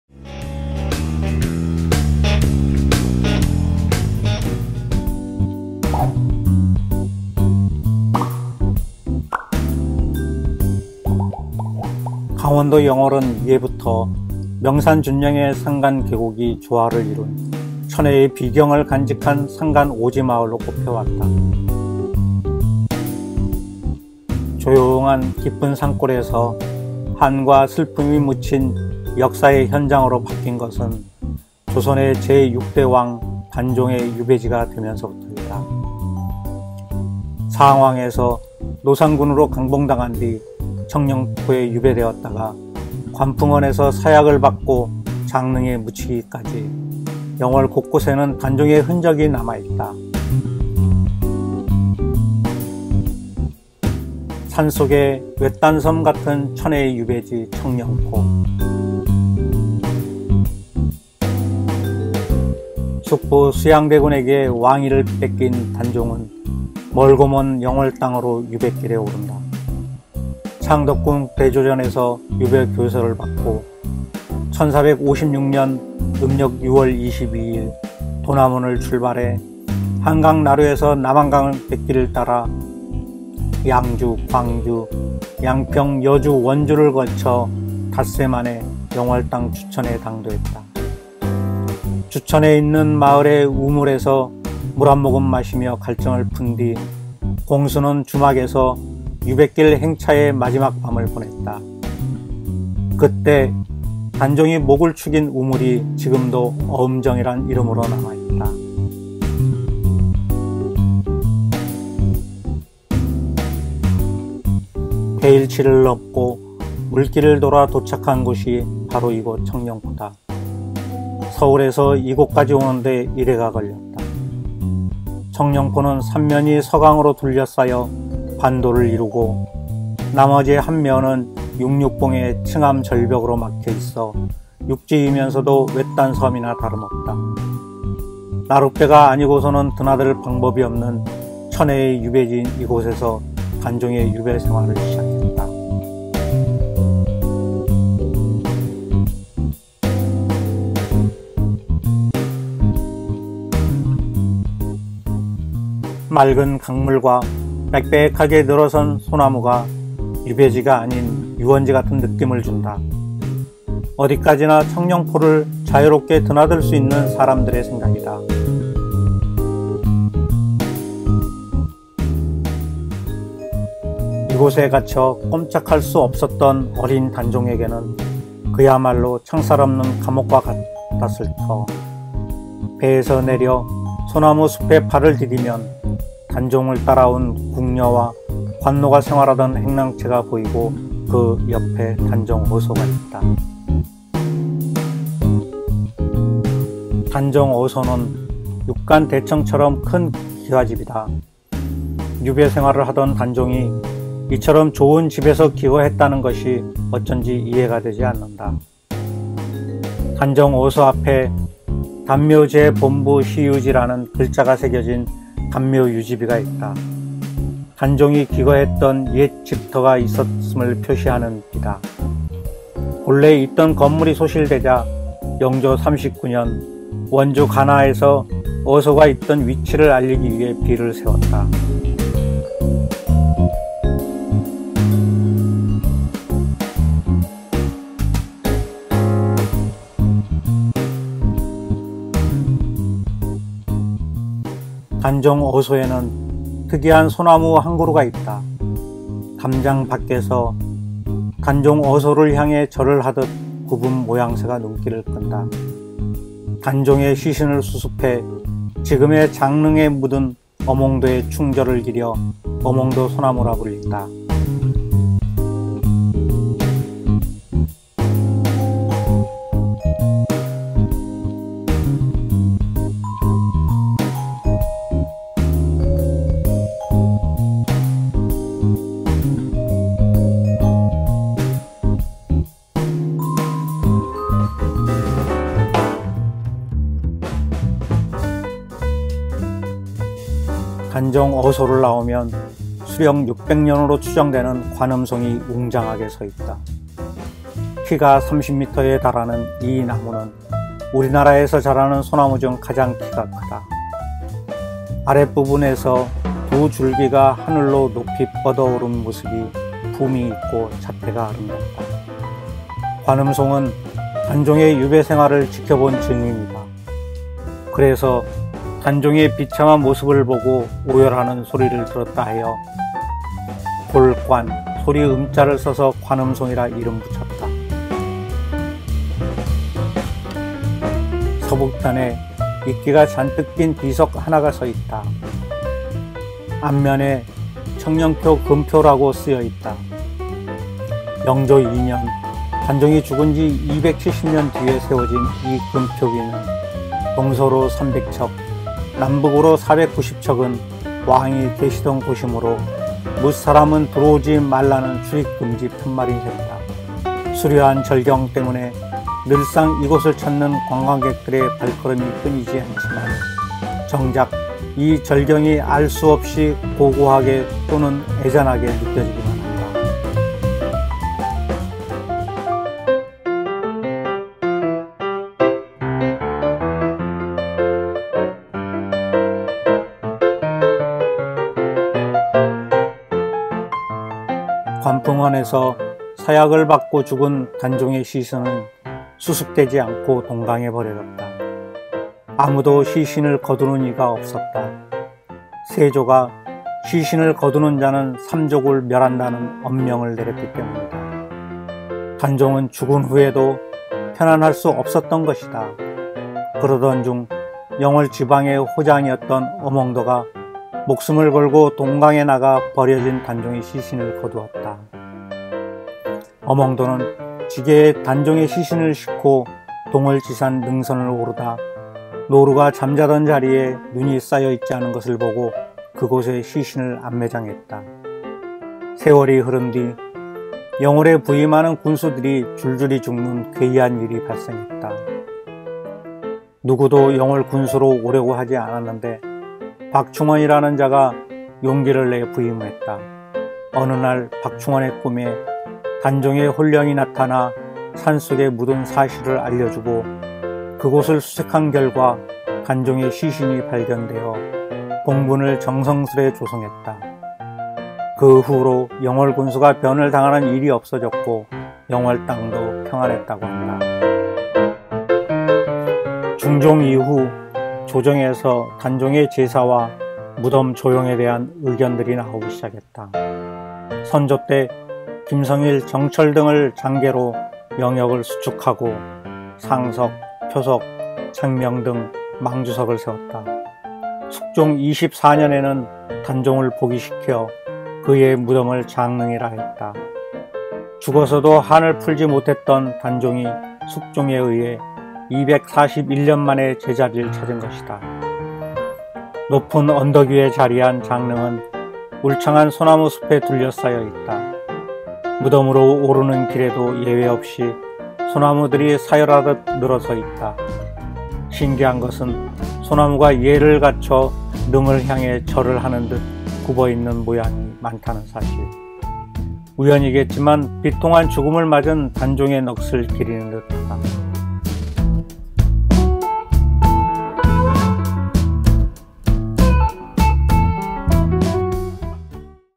청령포에 흐르는 눈물이 장릉에... - 자연의 감동이 있는 강원도 영월
강원도 영월은 예부터 명산준령의 산간계곡이 조화를 이룬
천혜의 비경을 간직한 산간 오지 마을로 꼽혀왔다.
그런데 깊은 산골에서 한과 슬픔이 묻힌 역사의 현장으로 바뀐 것은
조선의 제6대 왕 단종의 유배지가 되면서부터다.

상왕에서 노산군으로 강봉당한 뒤 청령포에 유배되었다가
관풍헌에서 사약을 받고 장릉에 묻히기까지 영월 곳곳에는
단종의 흔적이 남아 있다.

하늘에서 본 아름다운 우리의 풍경
You can see beautiful scenery of Korea photographed with drones.